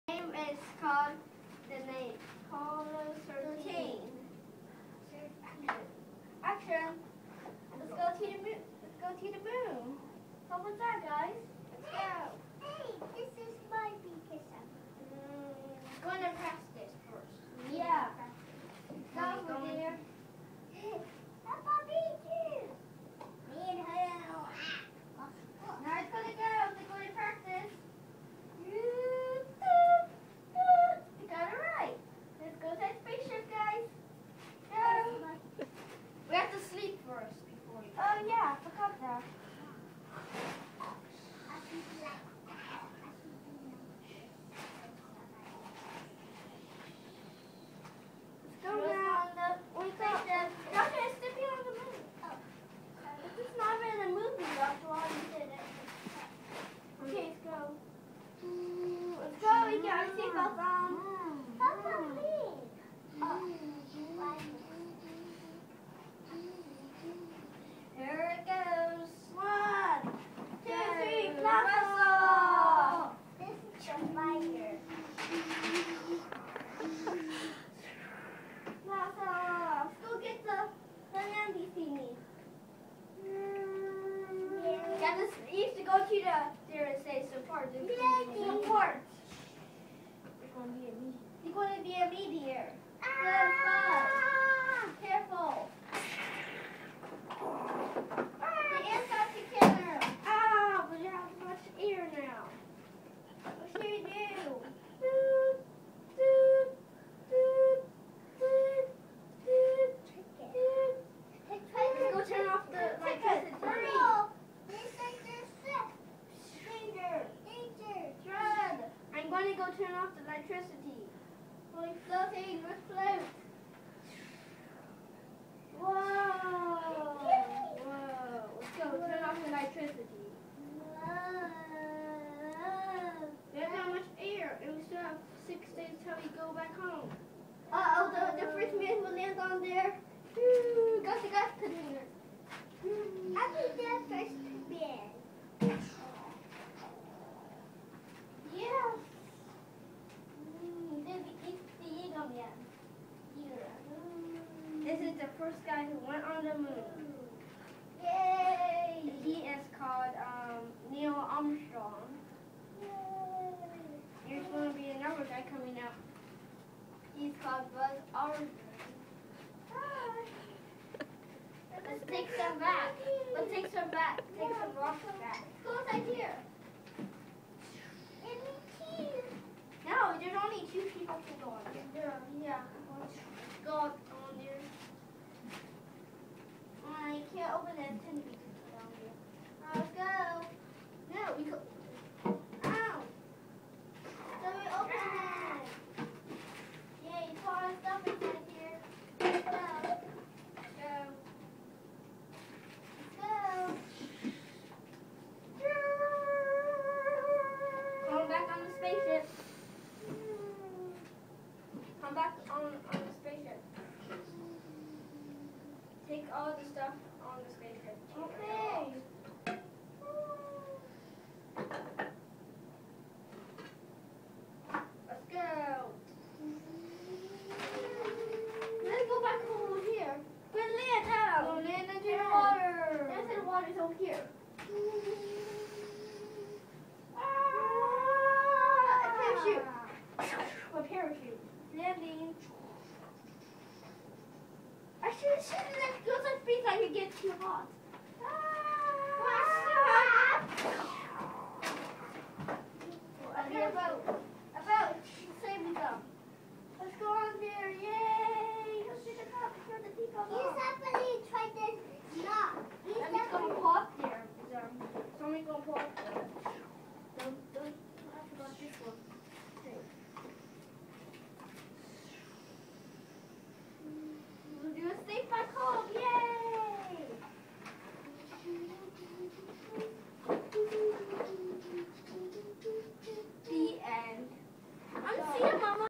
Apollo 13 mission recreated in our living room
Apollo 13 recreated with teddy bears and packaging.